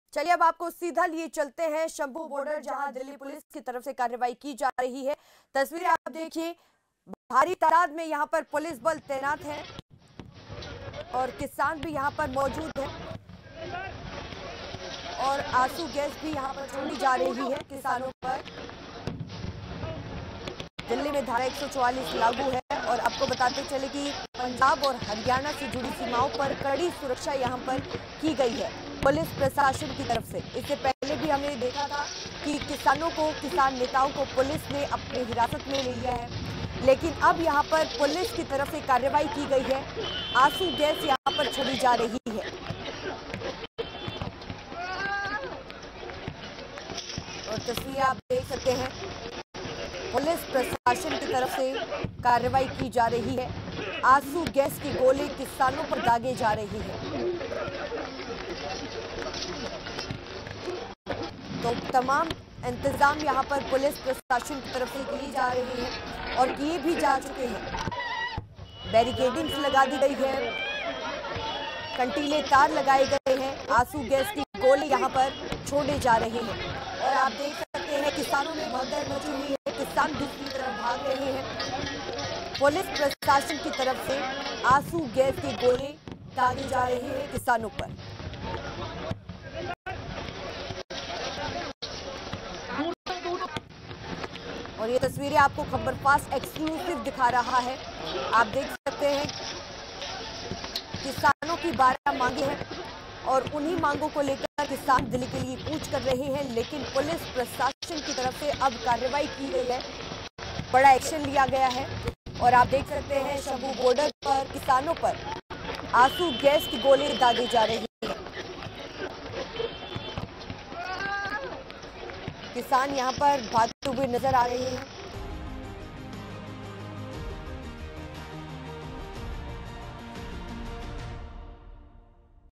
Farmers Protest Update: Shambhu Border पर किसानों को रोकने के लिए दागे गए आंसू गैस के गोले
किसानों ( Kisan Andolan ) ने MSP समेत खेती से जुड़ी अपनी मांगों को लेकर एक बार फिर से दिल्ली ( Delhi ) मे डेरा डालने की तैयारी कर ली है. दिल्ली कूच के लिए निकले किसान अंबाला (Ambala) से होते हुए पंजाब हरियाणा के शंभू बॉर्डर(Shambhu Border) पहुंच गए है. शंभू बॉर्डरपहुंचते ही पुलिस ने किसानों पर आंसू गैस (Tear Gas) के गोले दागे. जिससे यहां भगदड़ मच गई.